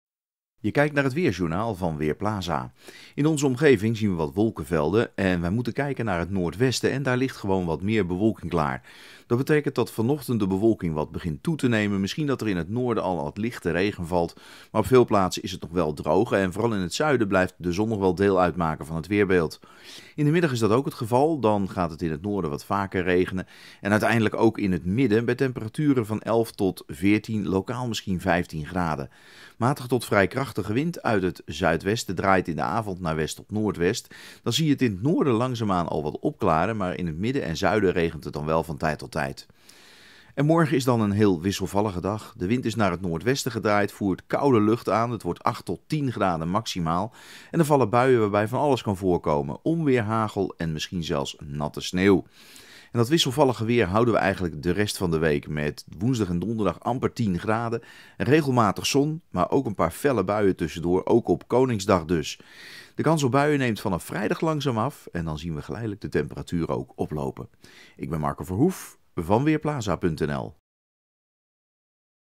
Weerbericht maandagochtend: Meer wolken en wat regen
Vandaag gaat het vanuit het noorden op steeds meer plaatsen regenen. Pas in de avond bereikt de regen het zuiden van het land. Het wordt 10 tot 15 graden en de zuidwestenwind is matig tot vrij krachtig. Morgen valt er eerst regen in het zuiden, later vallen er overal buien met kans op hagel en een klap onweer. In het noorden is ook natte sneeuw mogelijk. Het wordt 8 tot 10 graden. Woensdag verandert er weinig. Op Koningsdag vallen er buien, opnieuw met kans op hagel en een klap onweer bij maxima rond 10 graden. 's Avonds is het waarschijnlijk droog.